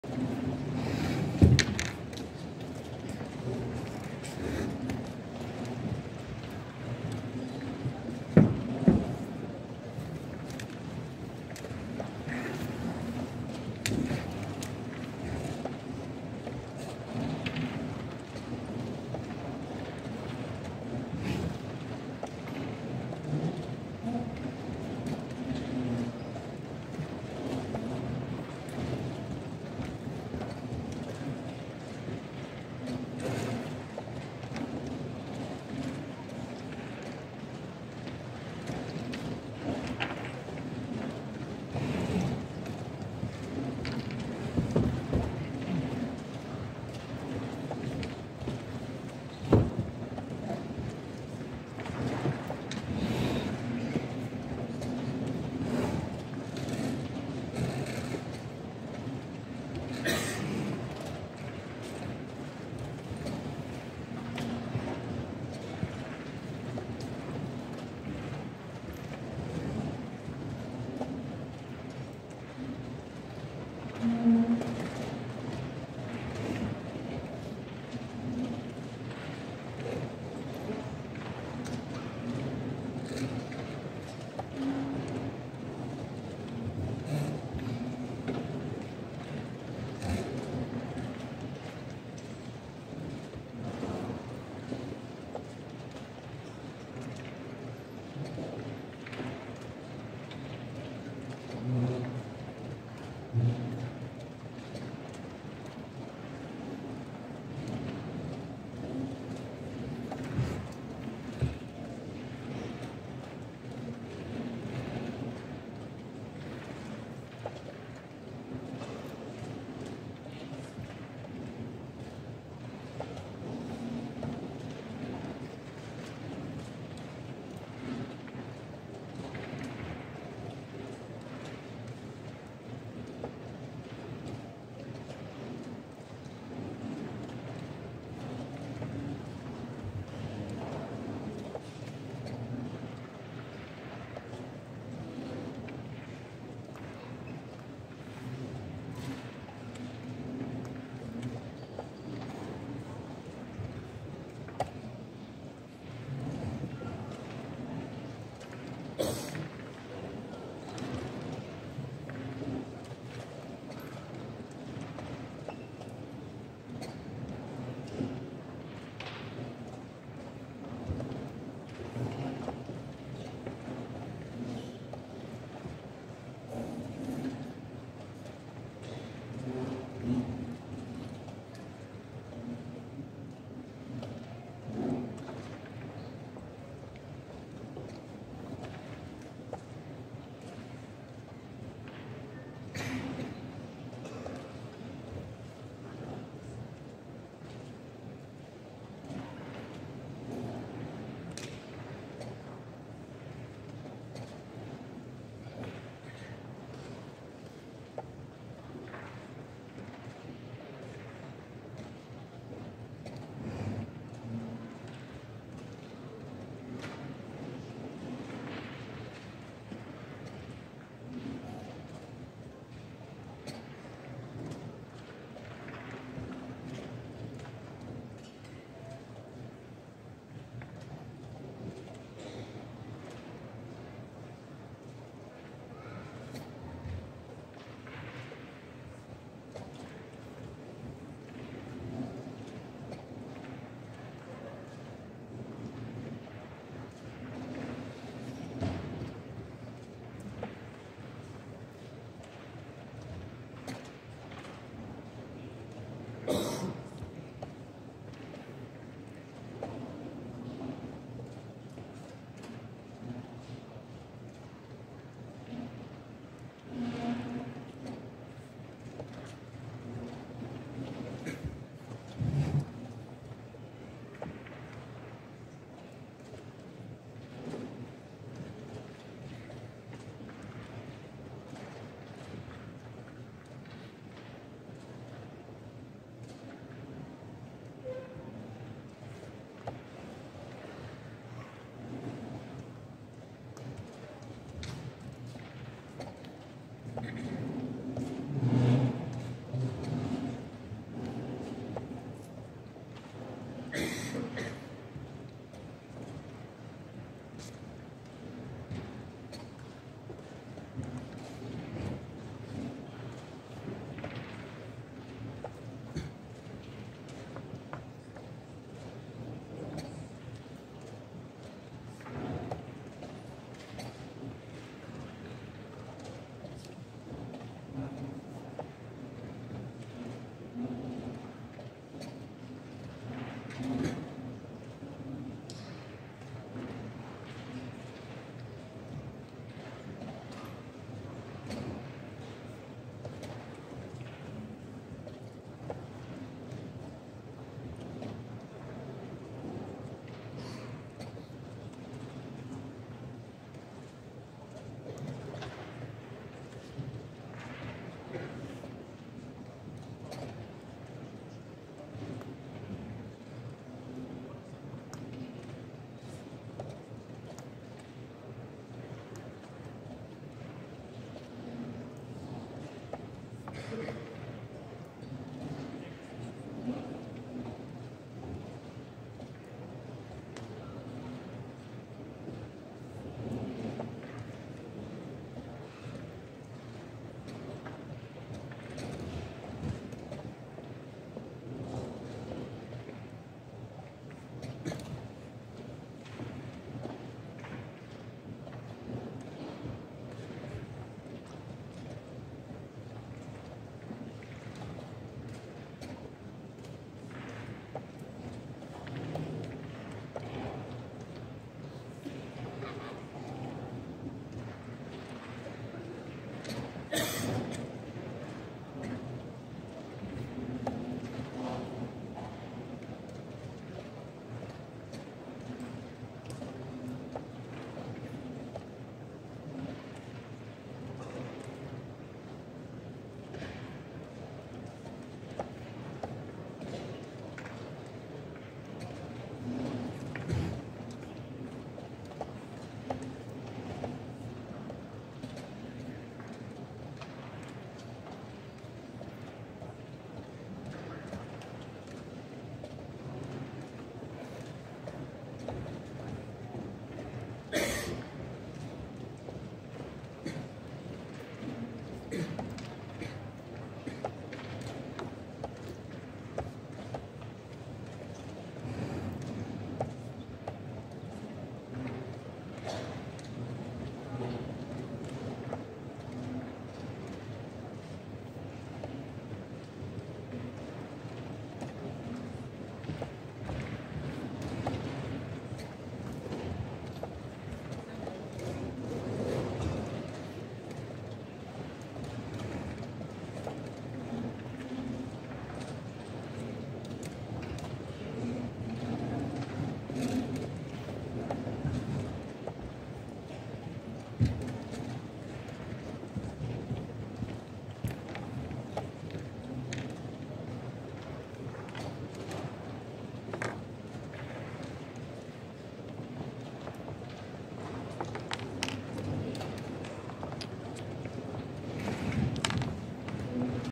GM Hannes Stefansson - GM Svane Rasmus | European Championships
European Individual Blitz and Rapid Championships 2022, Poland, Katowice